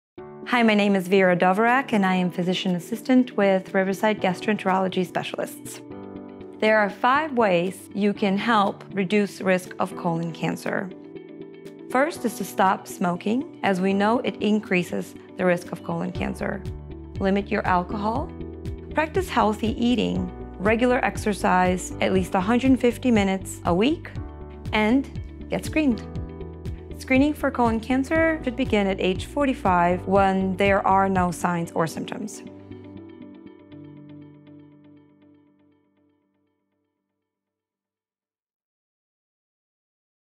Riverside Health - 5 Ways to Prevent Colon Cancer
What are ways I can prevent colon cancer? You may be able to lower your risk of developing colorectal cancer with some simple healthy lifestyle changes. Vera Dovirak, P.A., shares five ways you can prevent colon cancer.

• Alcohol/Tobacco – Limit alcohol use to no more than two drinks a day for men and one drink a day for women. Stop using tobacco products.
• Diet – Eat healthy foods. Low-fiber foods, high-fat diets: diets lacking fruit and vegetables, as well as those heavy in red meat and processed meat (lunch meat, bacon, sausage) are linked to colon cancer.
• Exercise – Be active. Lack of physical activity increases the risk of cancer. The American Cancer Society recommends at least 150 minutes of moderate intensity or 75 minutes of vigorous intensity activity each week (or a combination of these), spread throughout the week.
• Weight – Maintain a healthy weight. Being overweight or obese raises your risk of colon cancer. A healthy weight can also prevent diabetes, which is a risk factor for colon cancer.
• Get Screened beginning at age 45.

Learn more about colon cancer prevention, screening, and more by visiting